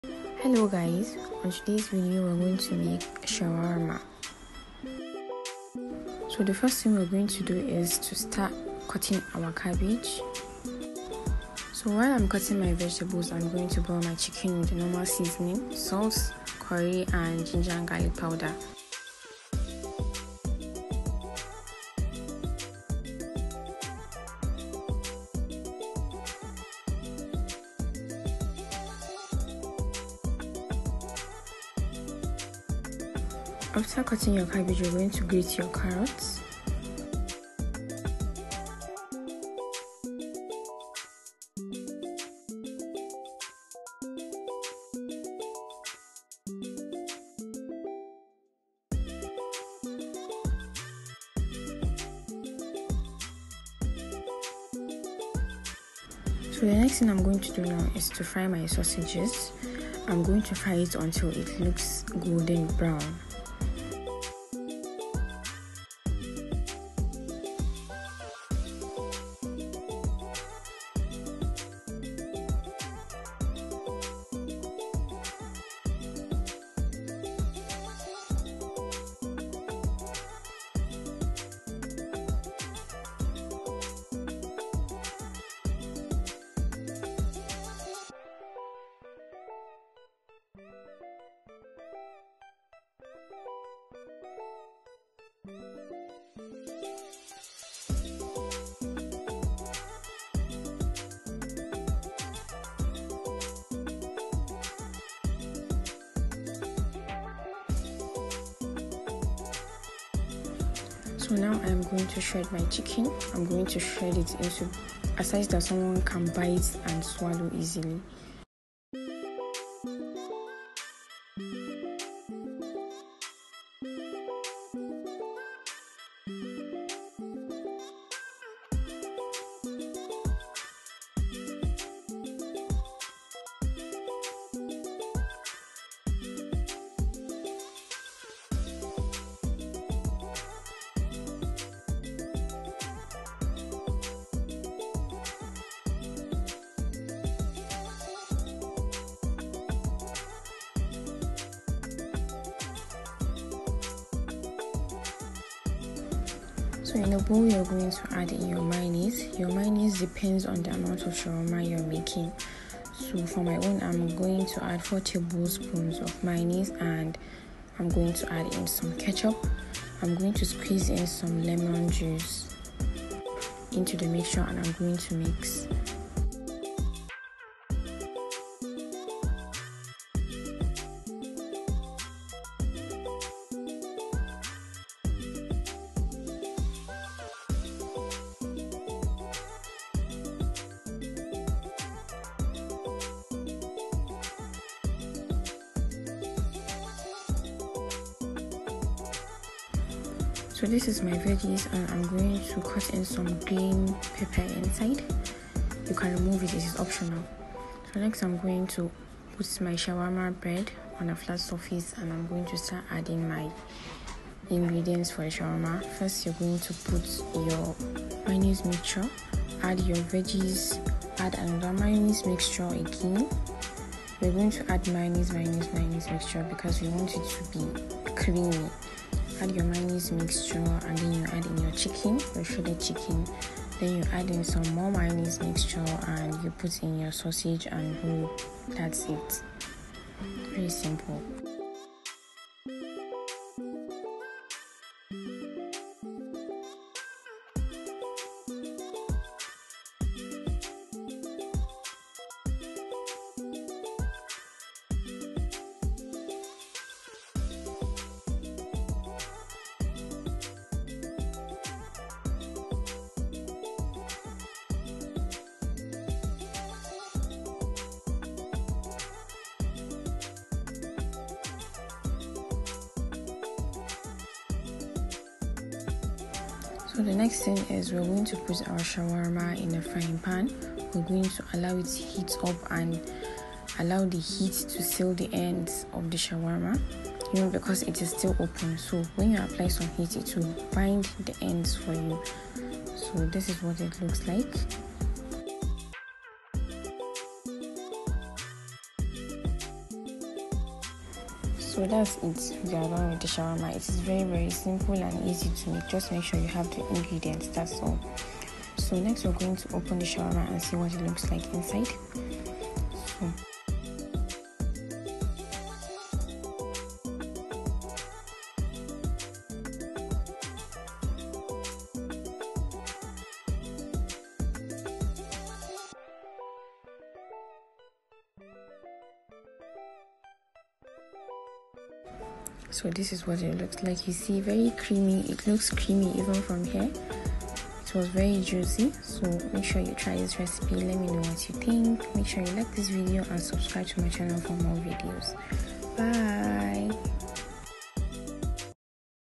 How to make chicken shawarma *Homemade*
Ingredients

shawarma bread
sausages (optional)
chicken (cooked)
Sherreded cabbage
carrots
green bell peppers (optional)
mayonnaise
ketchup
sweet chilli sauce (optional)
lemon juice

Make sure you try this recipe and let me know what you think. If you have any questions you can ask me in the comments section. Bye.